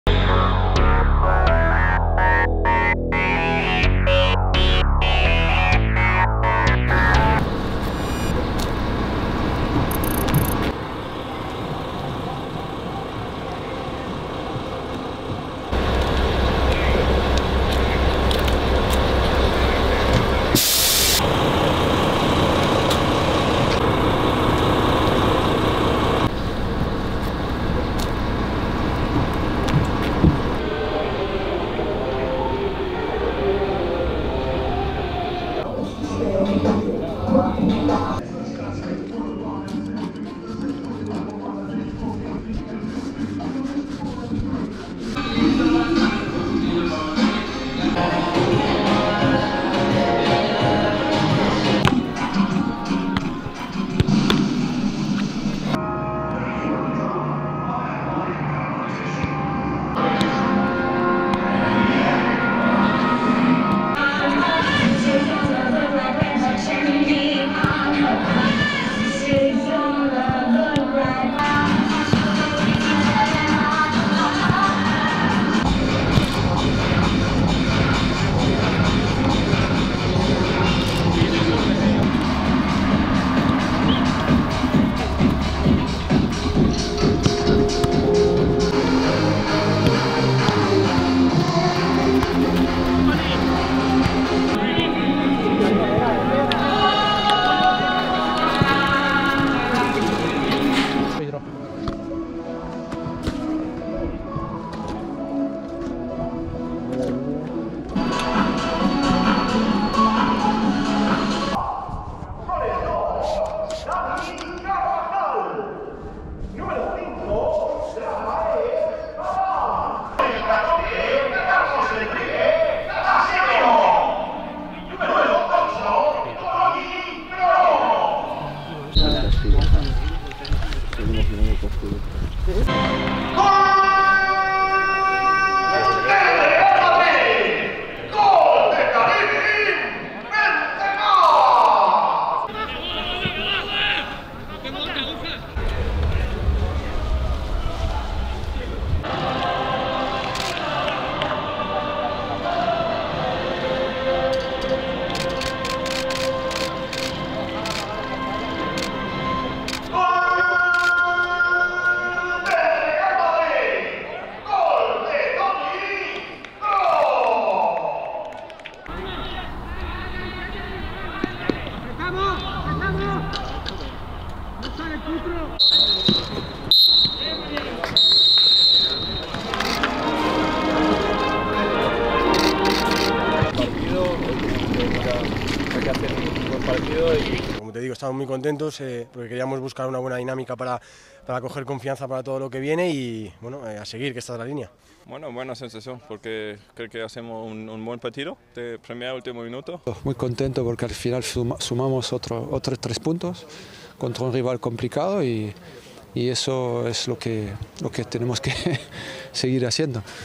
🦇 BEHIND THE SCENES | Real Madrid 2-0 Valencia | Benzema & Kroos seal win!
Real Madrid's run in LaLiga continues. This time the win came at the Di Stéfano against Valencia, with goals from Karim Benzema and Toni Kroos sealing all three points. The win makes it two out of two for Zidane's team in just one week, following the team's 2-0 win against Getafe. Thibaut Courtois also kept a clean sheet for the second match in a row, and will be looking to keep up the run as the team faces Real Valladolid away. Listen to Kroos, Nacho and Zidane speak post-match and more!

0:16 Leaving for the stadium
0:45 Warm-up
2:09 Kick-off
2:13 Real Madrid speaker announcing Benzema's goal
2:40 Real Madrid speaker announcing Kroos' goal
2:56 End of game
3:07 Nacho post-match
3:15 Kroos post-match
3:20 Zidane post-match